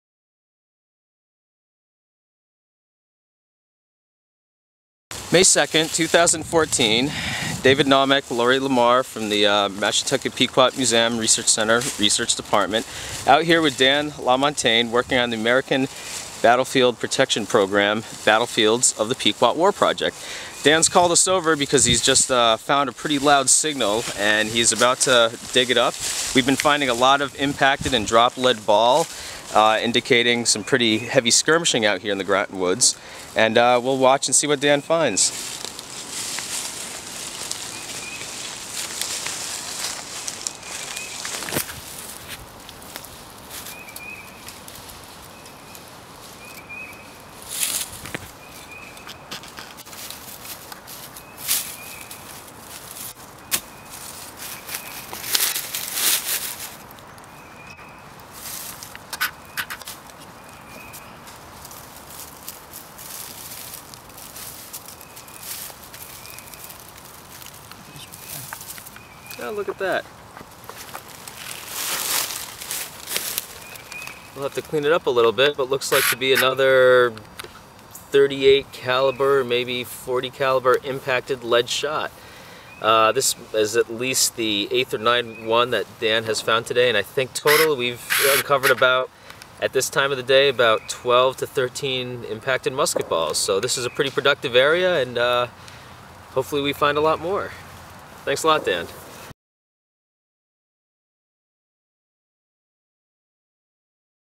Pequot Museum Locates a Battlefield Site -- In Process Archaeology
Watch the Pequot Museum's battlefield archaeologists unearth a historic 17th century musket ball in Groton, Connecticut. Current archaeology is documenting a battle between the Pequot versus English soldiers and their Native allies from May 26, 1637 -- after the events at Mistick Fort. The English had to retreat 6 miles through Mystic and Groton, where they were repeatedly counterattacked by the Pequot. This project is supported by the Pequot Museum, National Park Service American Battlefield Protection Program and University of Connecticut.